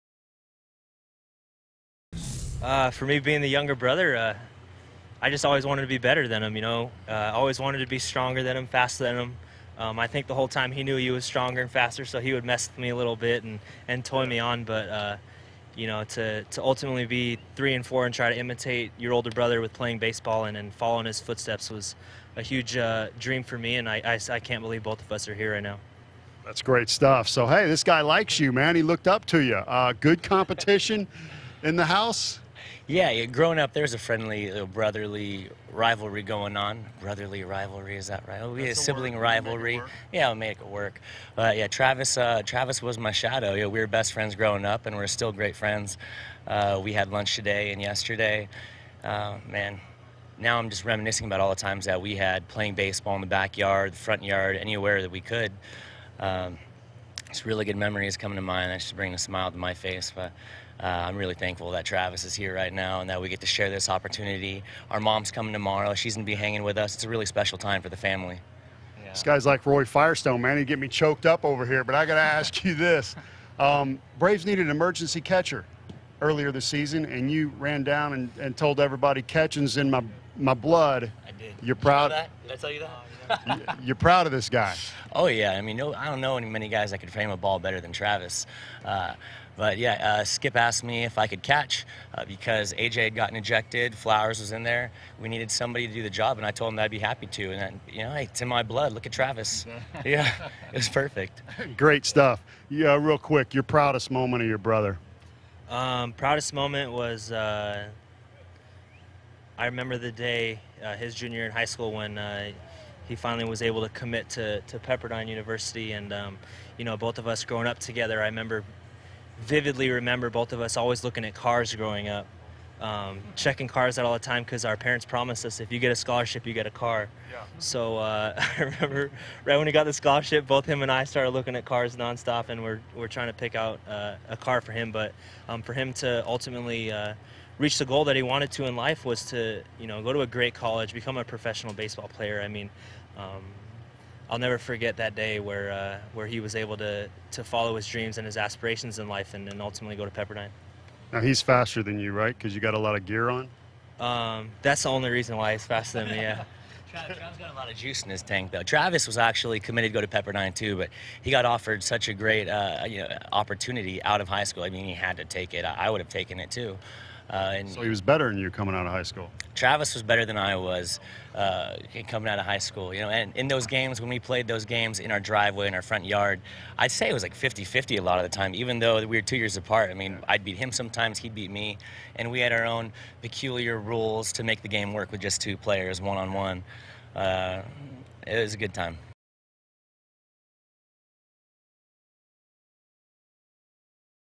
"It's a really special time for the family" -- d'Arnaud brothers on facing off
"It's a really special time for the family." Braves' Chase d'Arnaud and brother, Mets' Travis on playing each other